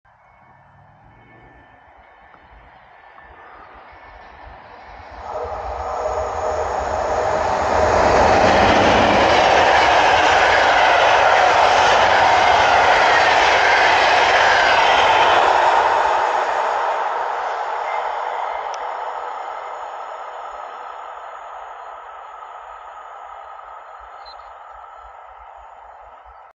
Pociąg PKP IC Intercity, okolica Poznań stacja Złotniki Grzybowe, Video Reframed 360 TRAIN
Insta360 ONE RS Video Reframed TRAIN - Pociąg PKP IC Intercity, okolica Poznań stacja Złotniki Grzybowe

Video dla zabawy i nauki Davinci Resolve :)